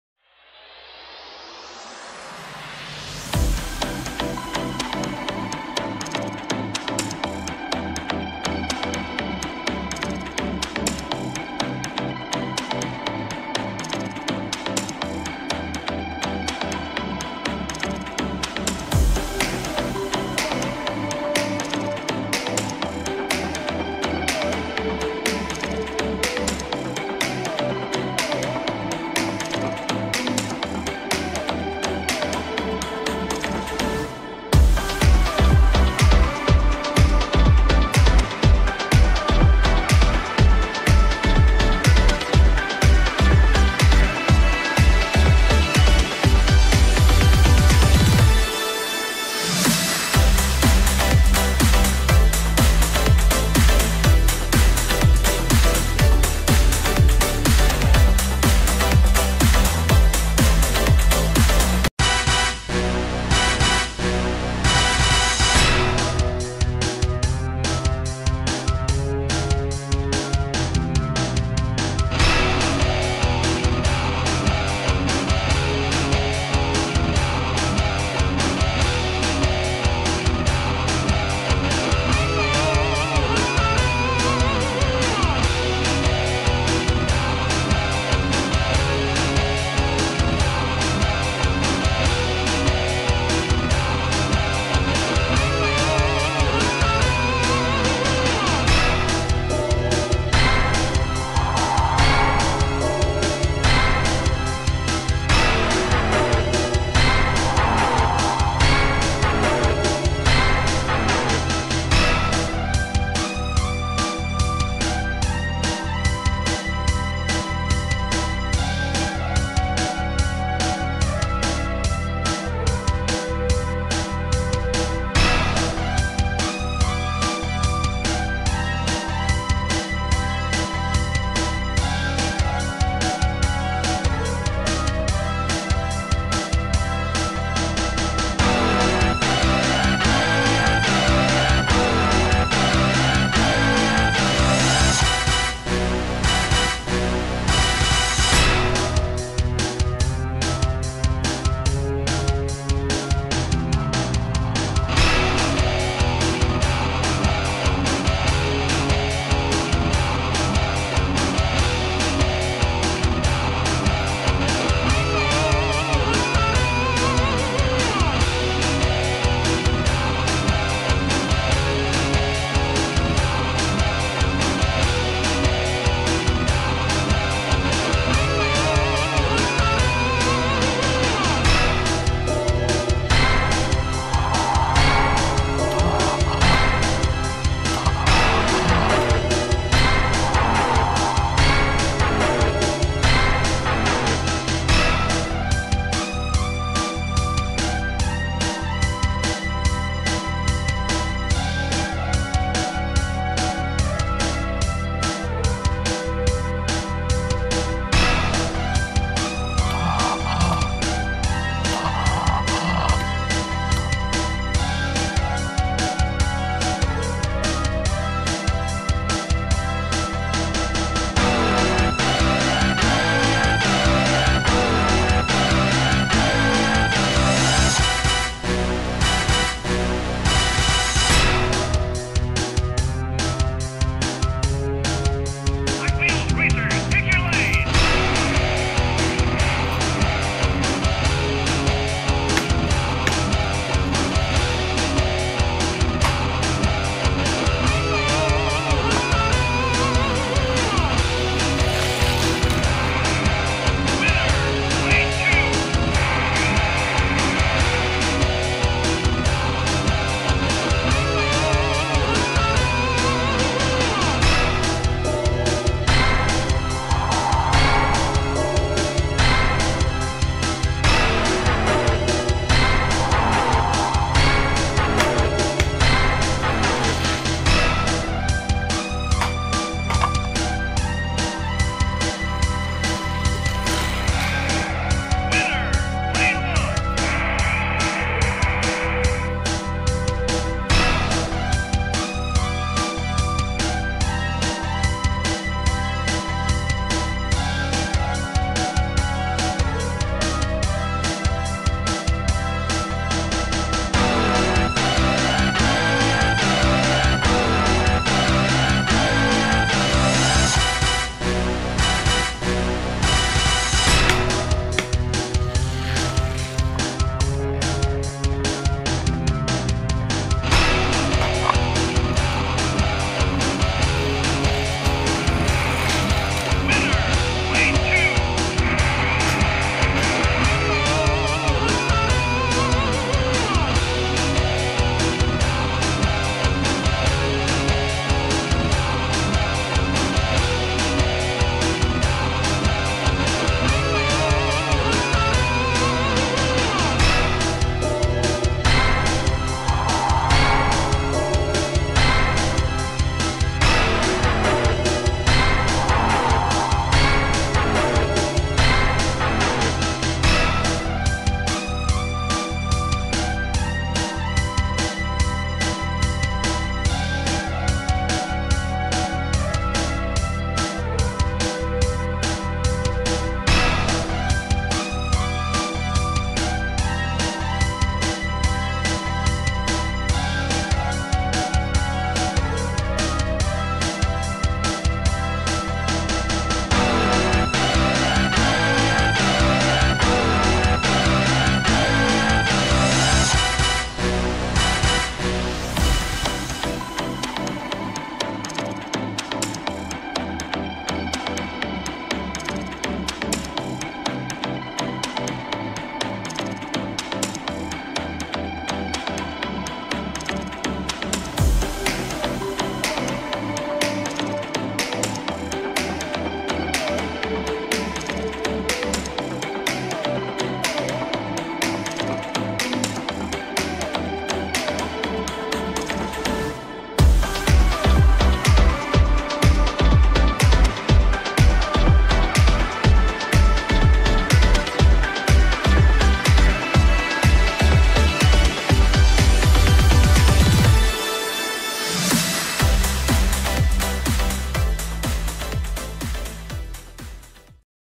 007 Drags
1/64th scale Drag Racing at Cobra Park International Dragway